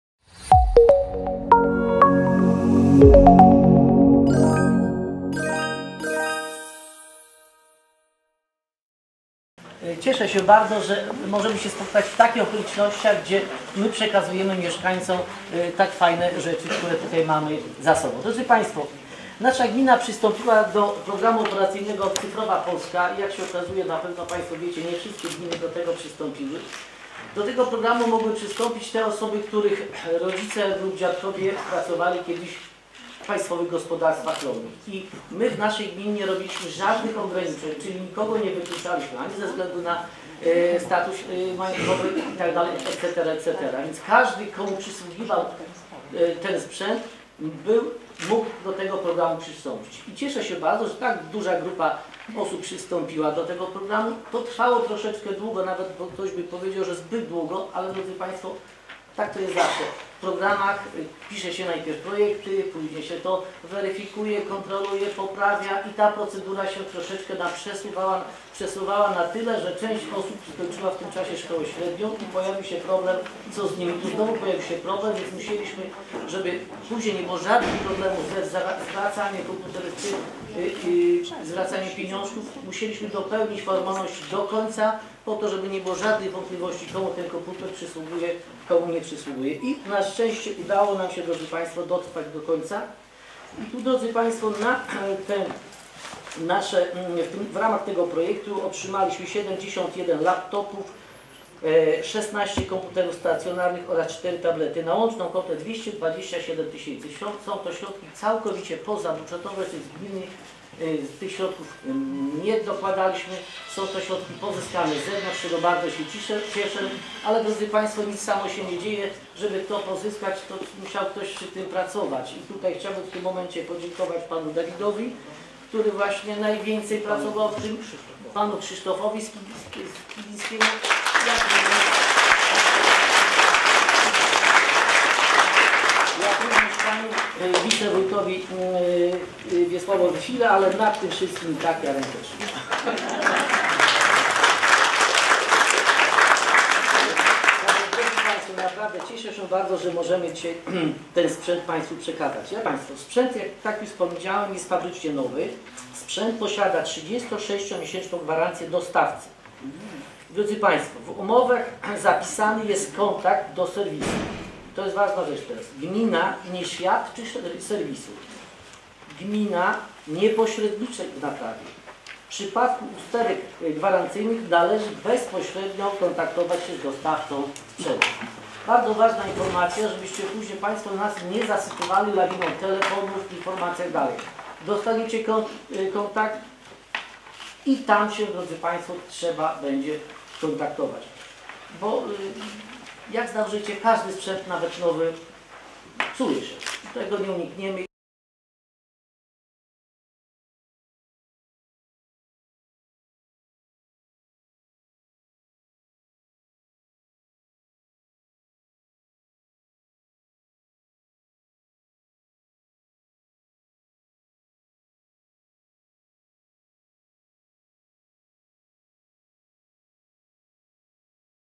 W Janowie przekazali sprzęt komputerowy rodzinom byłych pracowników PGR
21 października w Janowie Podlaskim odbyło się przekazanie sprzętu komputerowego zakupionego z dotacji z Programu Operacyjnego Polska Cyfrowa na lata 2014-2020, w ramach projektu grantowego „Wsparcie dzieci z rodzin pegeerowskich w rozwoju cyfrowym - Granty PPGR".